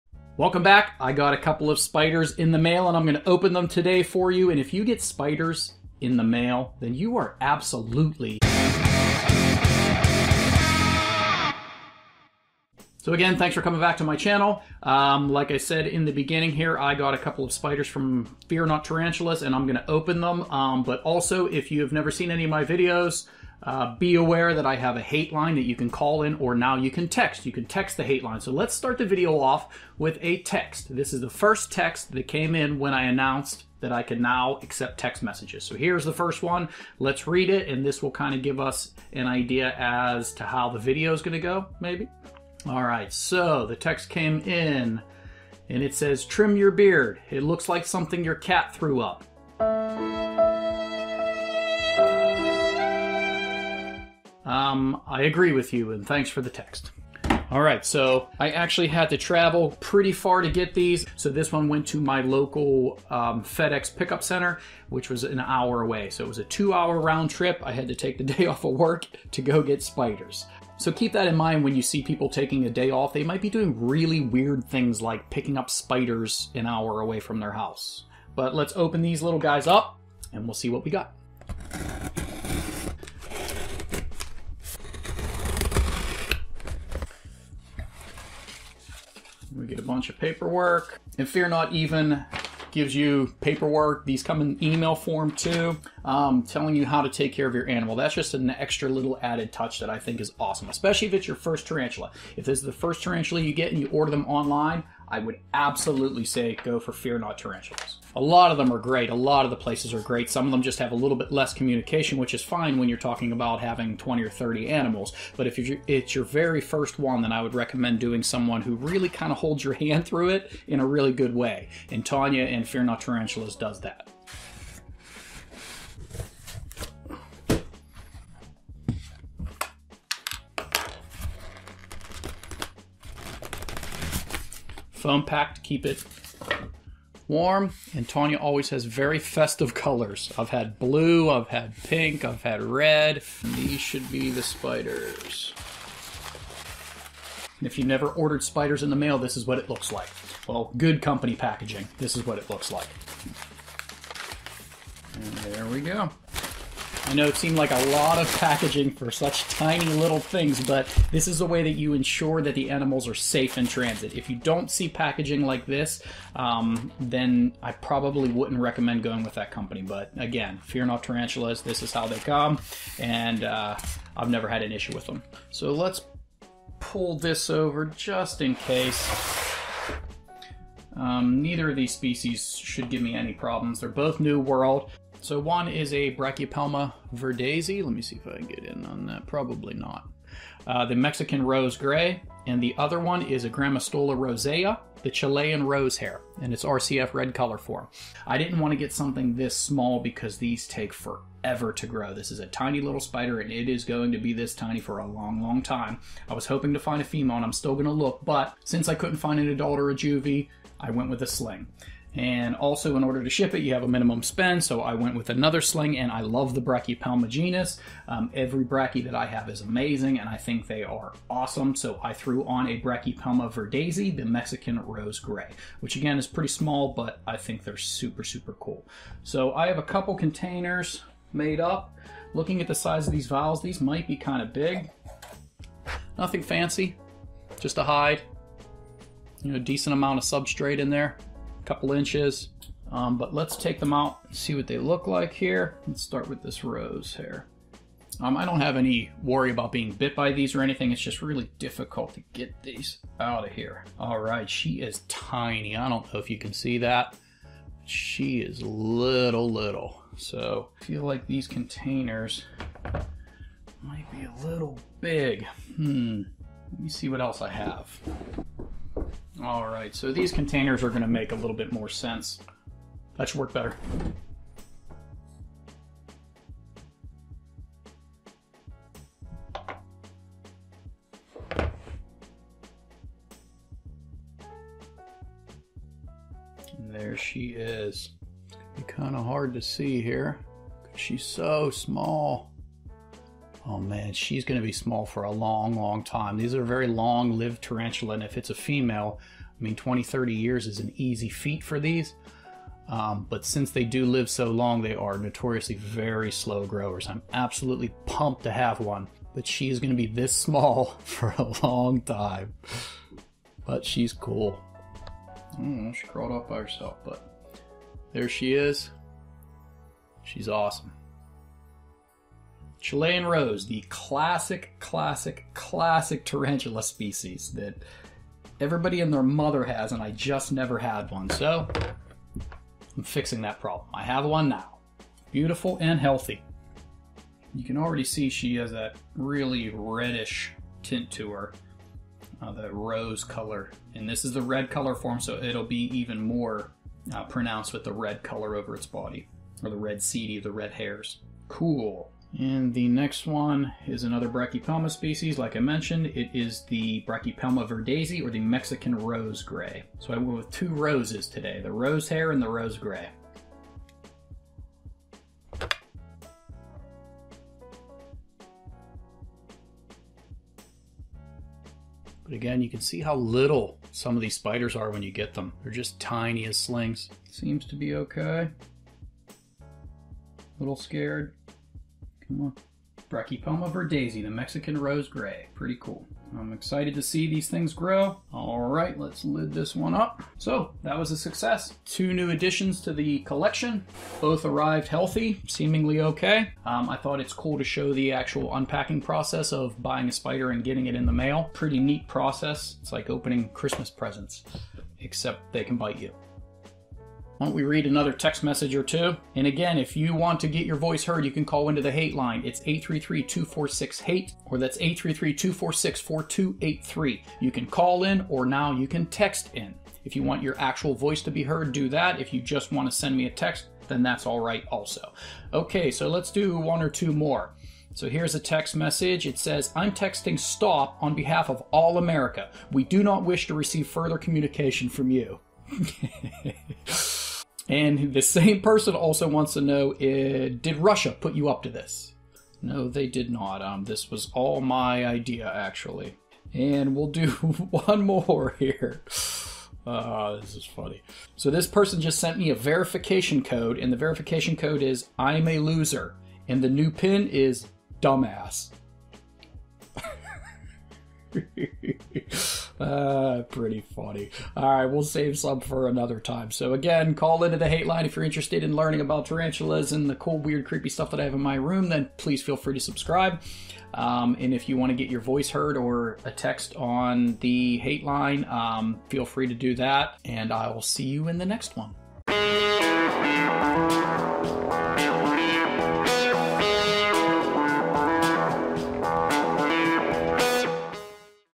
More TARANTULAS in the MAIL - Reading MEAN TEXTS! - Unboxing
Let's do a tarantula unboxing and read some mean text messages!

Call or text the "Hate-Line" now and leave me a message!  PLEASE BE AWARE!!!  If you call or text and leave a message.. you are giving me YOUR PERMISSION to use it in my videos... Please specify in the voice message if you DO NOT want it to be played online.